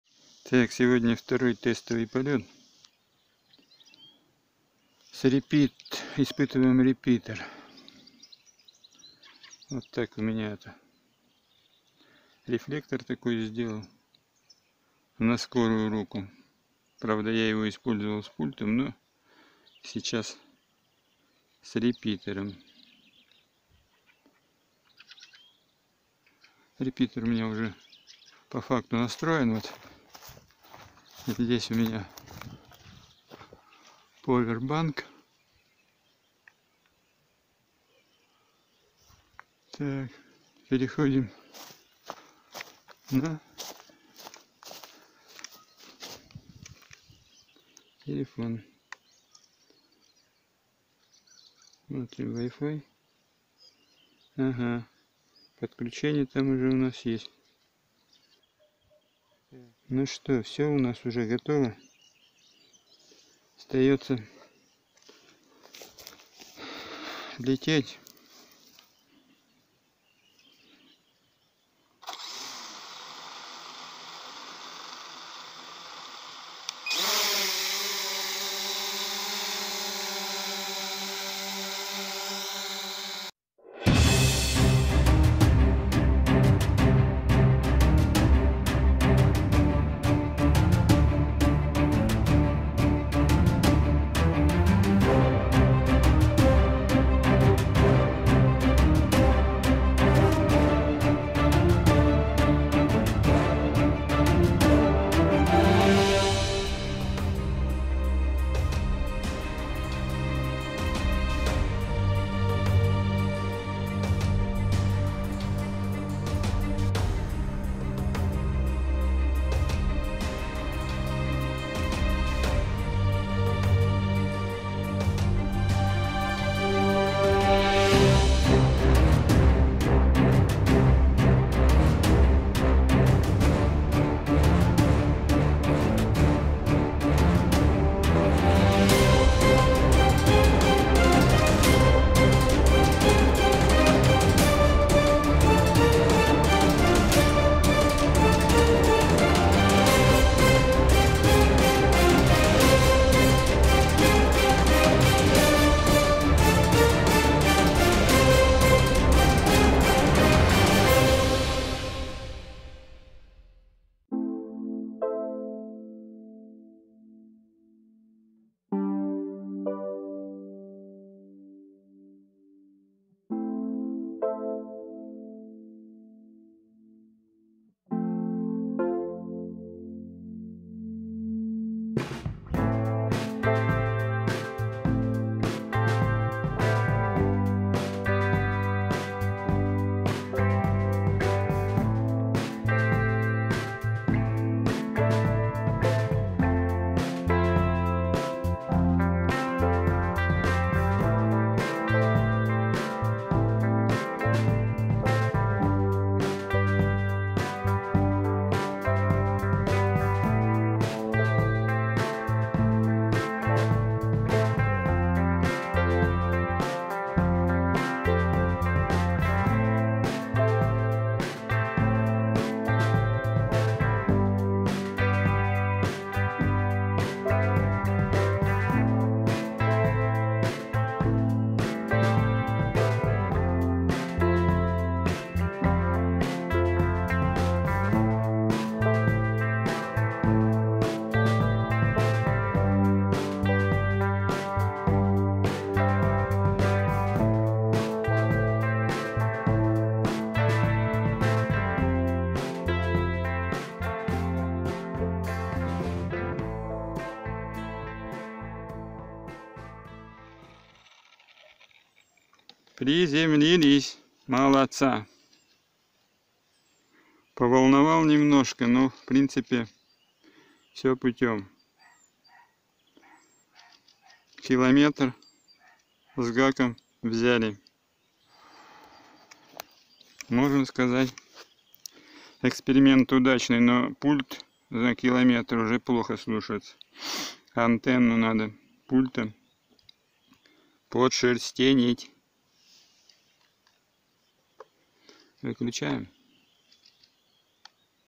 Дрон SJRC F11 - полет на дальность видеолинка 1119 метров с помощью репитера Comfast CF-WR371AC
Показан тестовый полет на дальность видеолинка 1.1 км с помощью репитера Comfast CF-WR371AC на квадрокоптере SJRC F11...